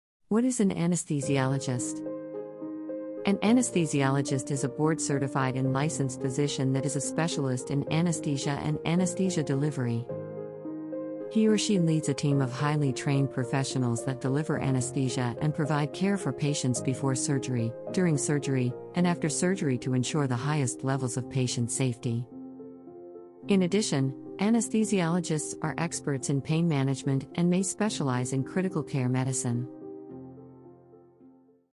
What is an Anesthesiologist
What is an Anesthesiologist - A Short Definition

An Anesthesiologist is a board certified and licensed physician that is a specialist in anesthesia and anesthesia delivery. He or she leads a team of highly trained professionals that deliver anesthesia and provide care for patients before surgery, during surgery, and after surgery to ensure the highest levels of patient safety. In addition, Anesthesiologists are experts in pain management and may specialize in critical care medicine.

We would like to thank the Author of this Medical specialty article Dr Callie Torres for writing the content used for this video